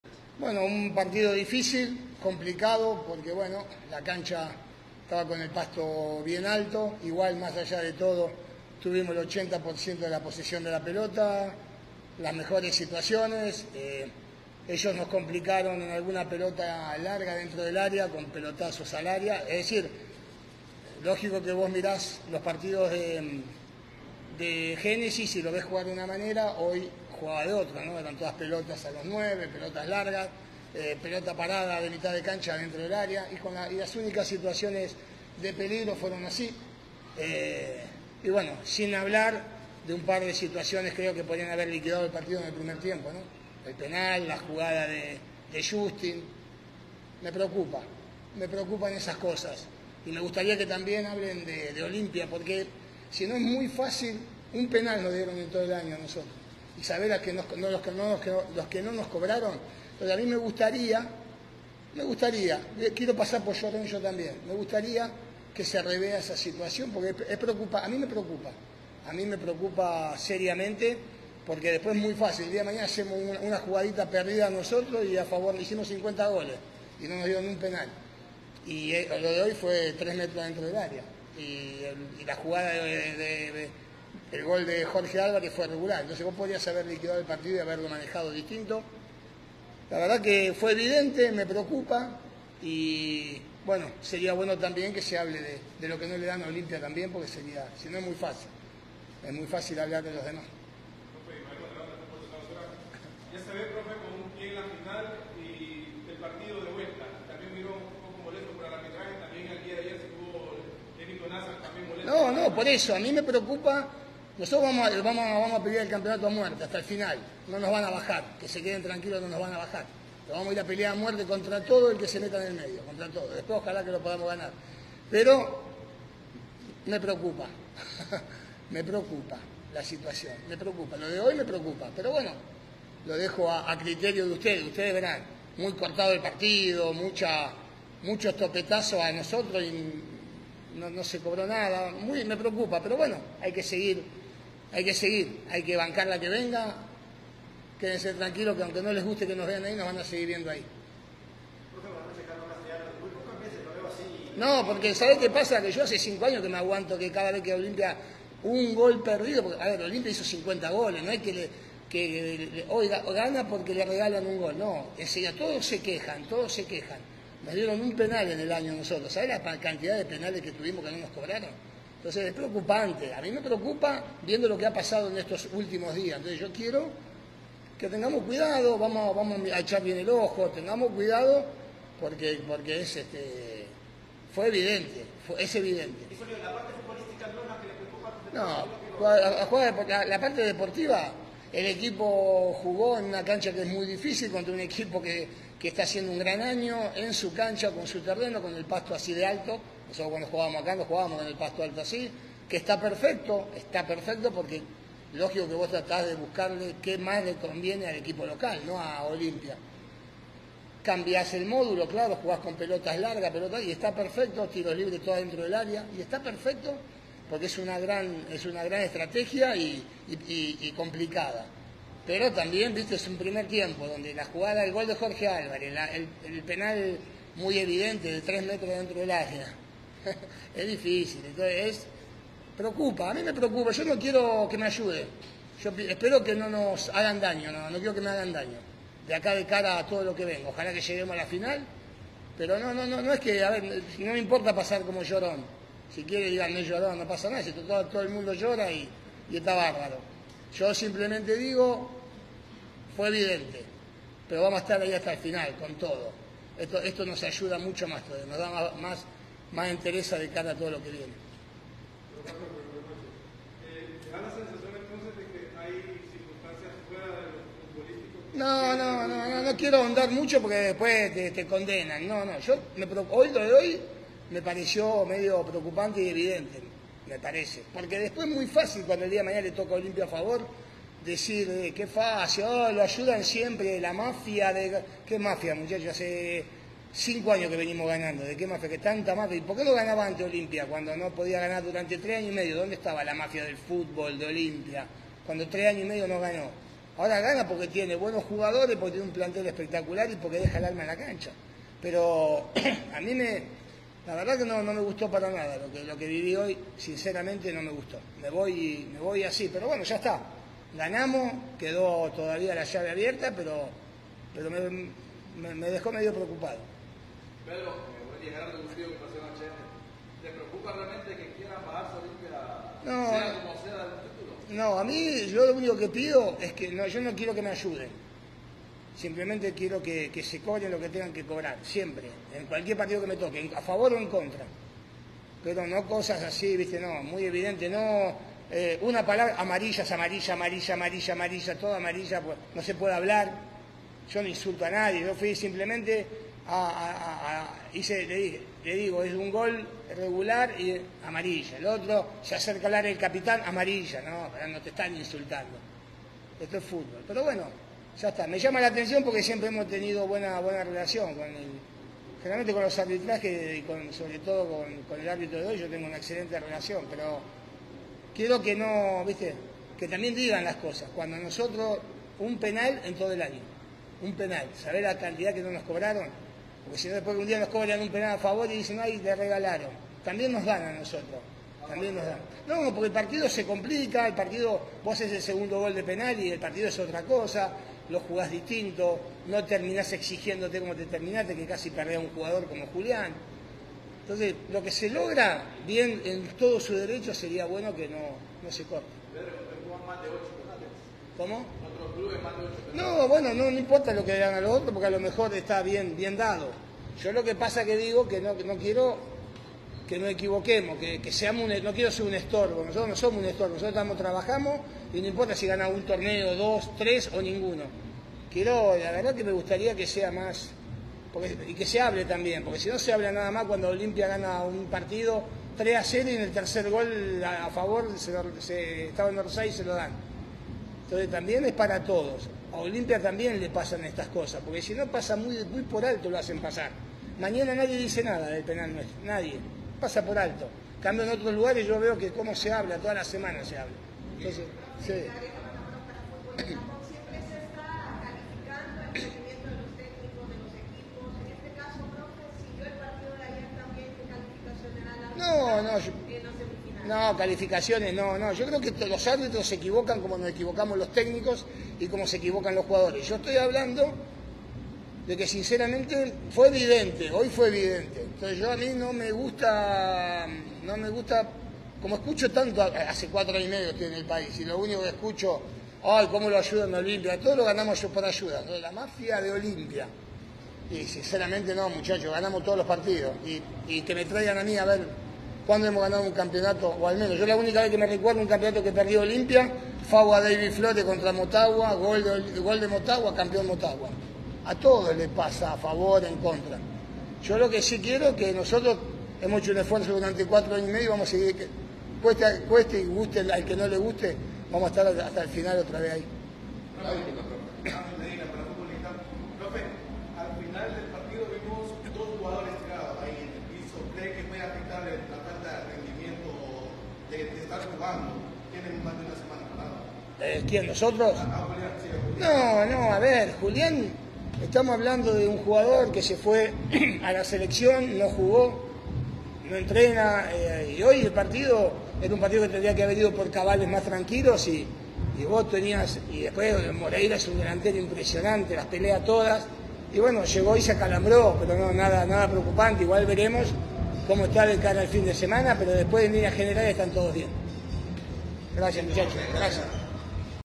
Declaraciones de Troglio contra Génesis
El argentino dejó todo contra el arbitraje de Melvin Matamoros y advirtió que van a la final y que no los van a bajar.